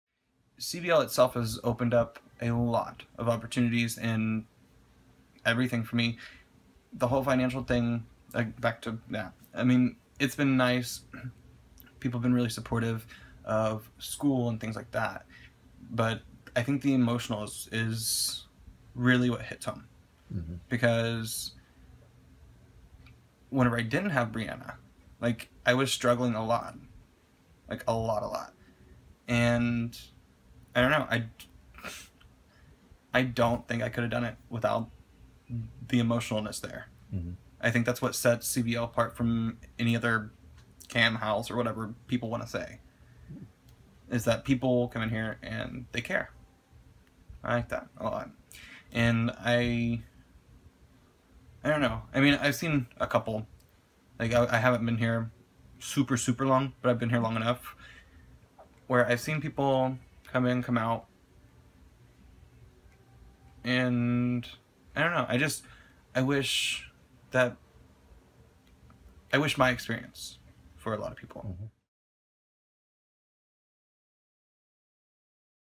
Kody explains being on the set of the gay real world
Kody, one of the current cast members explains what its like to be on the show